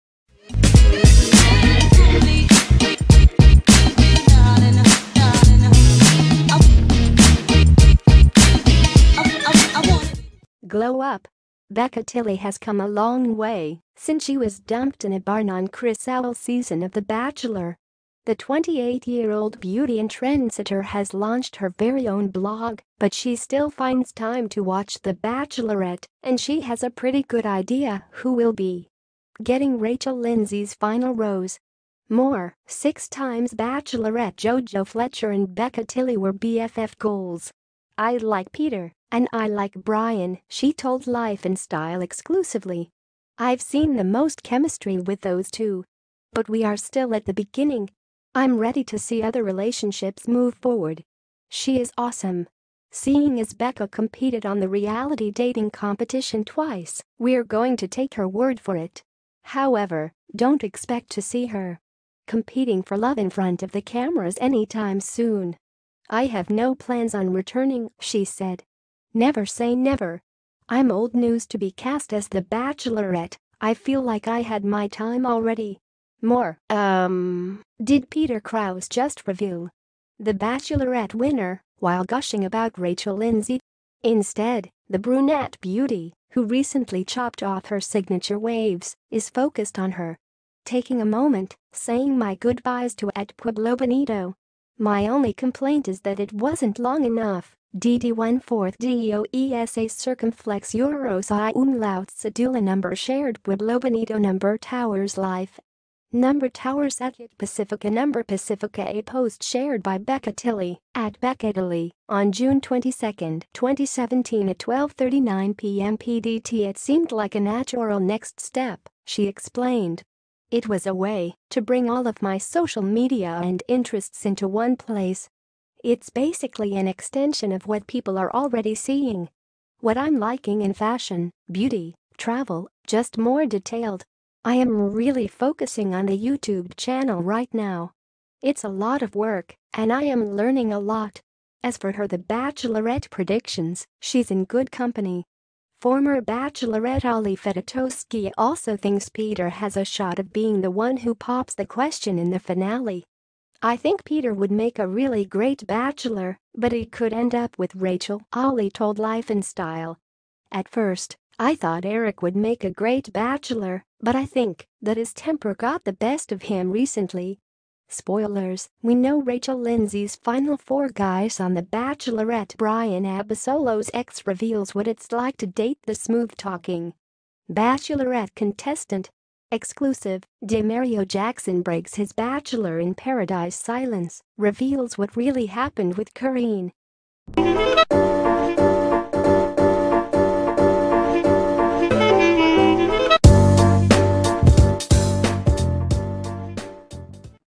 Bachelor' Star and Blogger Becca Tilley Predicts Who Will Get Rachel Lindsay's Final Rose!
Bachelor' Star and Blogger Becca Tilley Predicts Who Will Get Rachel Lindsay's Final Rose! (EXCLUSIVE). Glow up! Becca Tilley has come a long way since she was dumped in a barn on Chris Soules&#39; season of The Bachelor.

The 28-year-old beauty and trendsetter has launched her very own blog, but she still finds time to watch The Bachelorette  — and she has a pretty good idea who will be getting Rachel Lindsay&#39;s final rose.

MORE: 6 Times &#39;Bachelorette&#39; JoJo Fletcher and Becca Tilley Were BFF Goals!

"I like Peter and I like Bryan," she told Life & Style exclusively. "I&#39;ve seen the most chemistry with those two. But we are still at the beginning! I&#39;m ready to see other relationships move forward. She is awesome!"

Seeing as Becca competed on the reality dating competition twice, we&#39;re going to take her word for it! However, don&#39;t expect to see her competing for love in front of the cameras anytime soon.

"I have no plans on returning," she said. "Never say never! I&#39;m old news to be cast as The Bachelorette… I feel like I had my time already!"

MORE: Um… Did Peter Kraus Just Reveal &#39;The Bachelorette&#39; Winner While Gushing About Rachel Lindsay?

Instead, the brunette beauty, who recently chopped off her signature waves, is focused on her budding career as a blogger.

Taking a moment, saying my goodbyes to @pueblobonito! My ONLY complaint is that it wasn&#39;t long enough! 💃🏼🌊☀️

A post shared by  Becca Tilley (@beccatilley) on Jun 22, 2017 at 12:39pm PDT

"It seemed like a natural next step," she explained. It&#39;s basically an extension of what people are already seeing. What I&#39;m liking in fashion, beauty, travel — just more detailed! I am really focusing on the